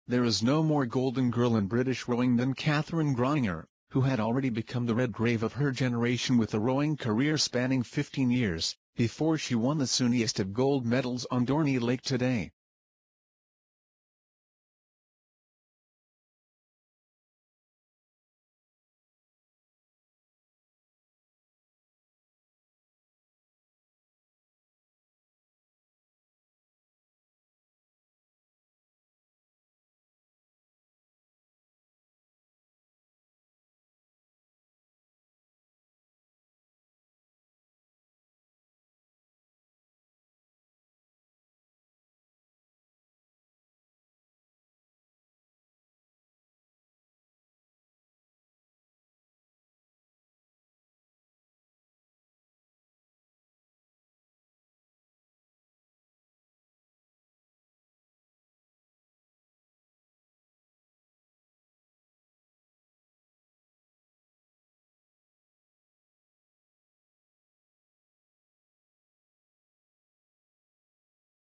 Katherine Grainger And Anna Watkins Meet Britain's Golden Girls Of Olympic Rowing
There is no more golden girl in British rowing than Katherine Grainger, who had already become the Redgrave of her generation with a rowing career spanning 15 years, before she won the sunniest of gold medals on Dorney Lake today.

 The criminal law postgraduate who moved from Scotland to Marlow, in Buckinghamshire, to focus on her sport had taken it up at Edinburgh University, having been dragged to a novice meeting of the boat club by a friend, and only staying out of bloodymindedness to prove she could make the top 16 cut. The rest, as they say is history.Breaking boundaries is not new to Grainger, who can claim Britain's first women's eights world medal, first women's Olympic medal, first Olympic women's sculling medal, and first singles medal.